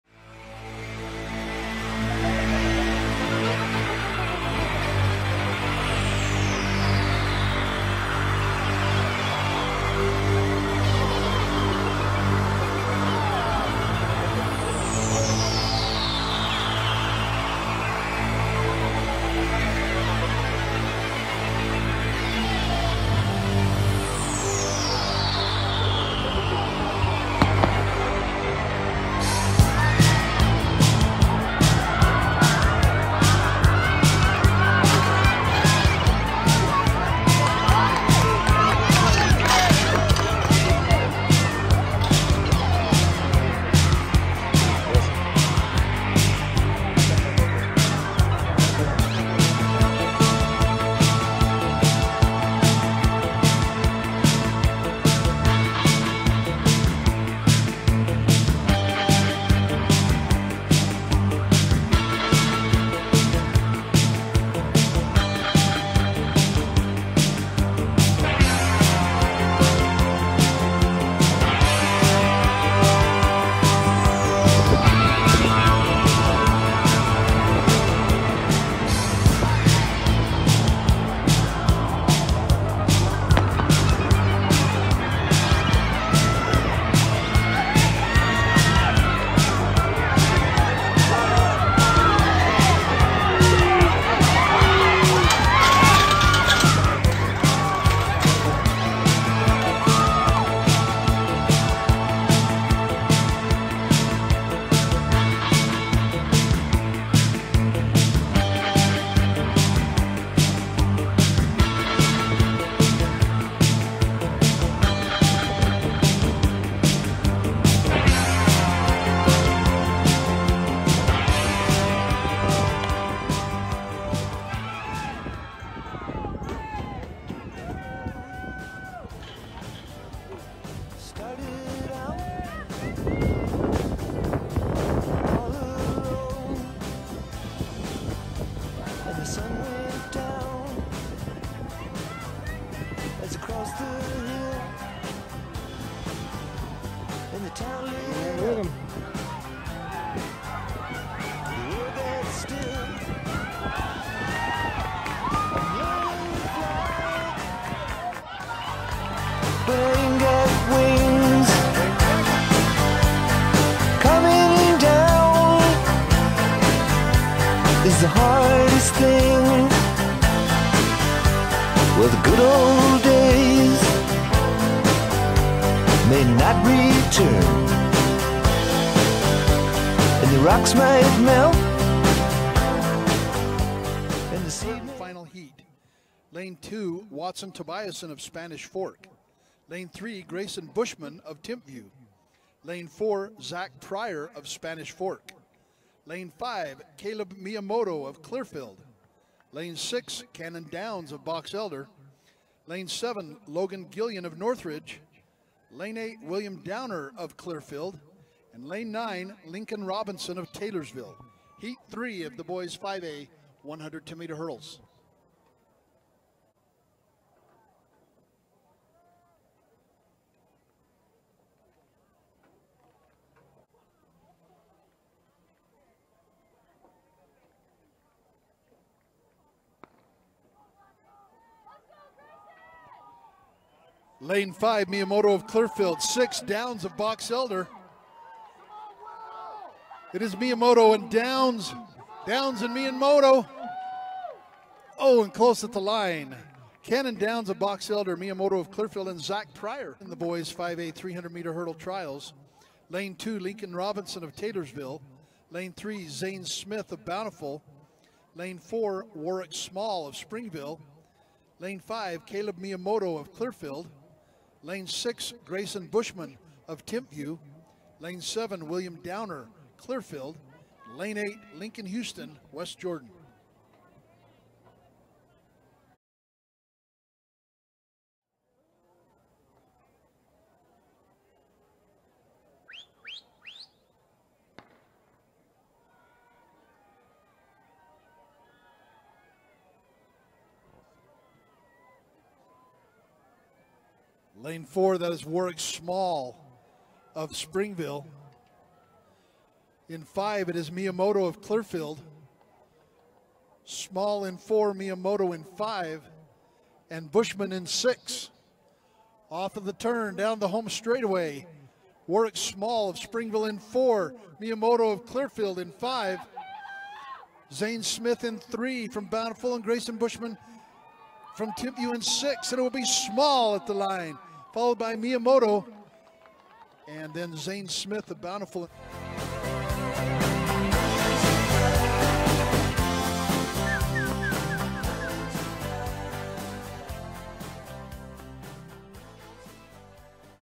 Lincoln’s 2024 Track & Field. Taylorsville high school track star.
Lincoln’s Utah state track meet. 5A hurdles. Region 4. ​⁠@nike